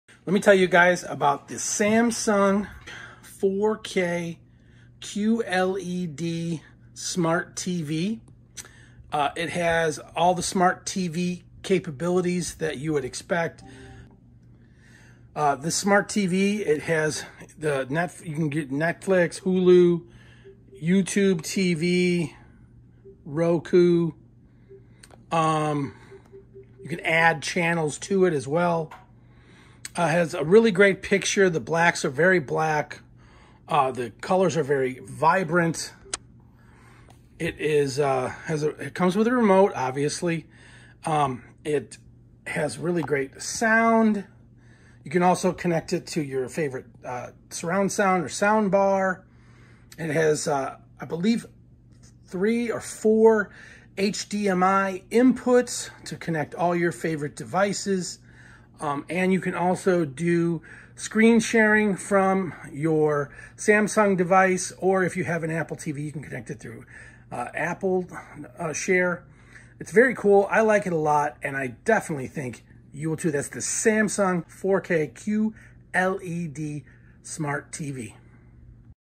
Samsung 4K QLED 75" Smart TV Product Review
This is a product review for the Samsun 4K QLED 75" Smart TV.  This smart tv is 75 inches wide, has deep blacks, and virbrant colors.  It have apps for all your favorite streaming providers.

Send Us Stuff:
BigdaddysWorld

Free Video Clip for News Report:
Video by Bellergy RC from Pixabay